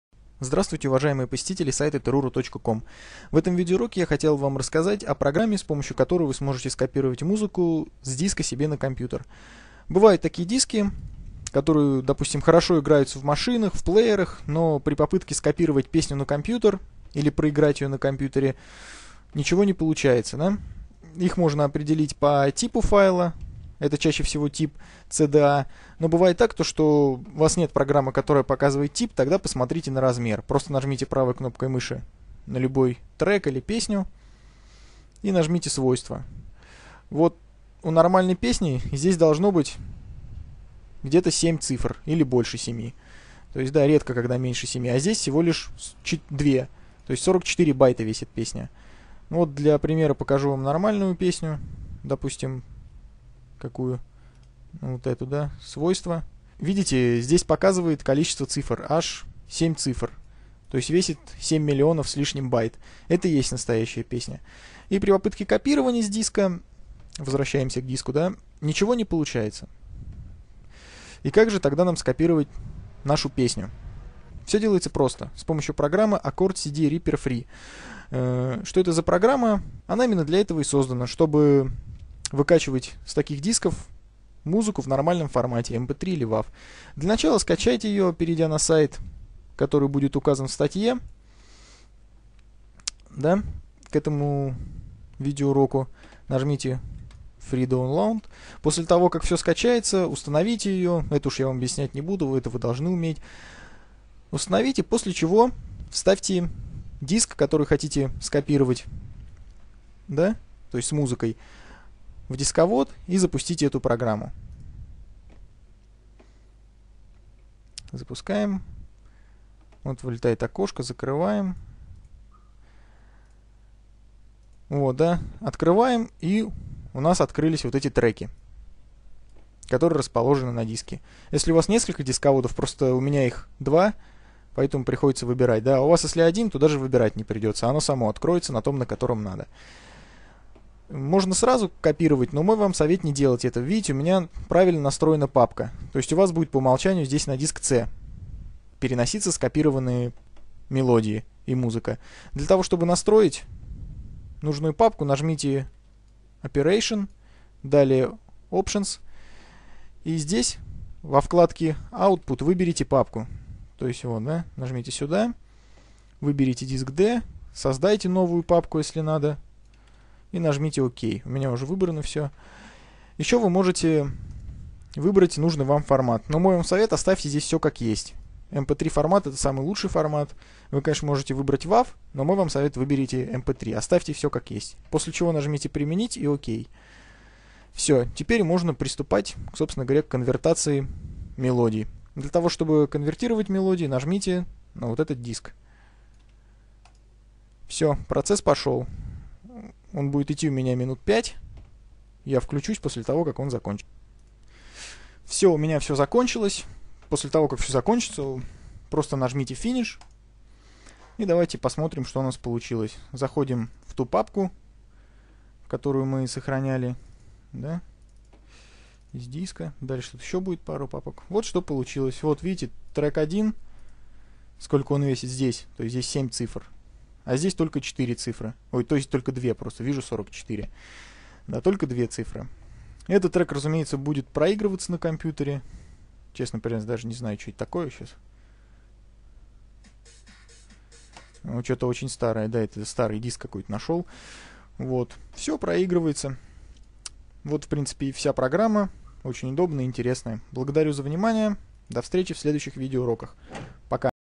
Конвертер cda в mp3. Программа для копирования дисков.
В данном видео уроке будет рассказанно о том, как и с помощью какой программы, скопировать музыку в формате cda, с диска на компьютер.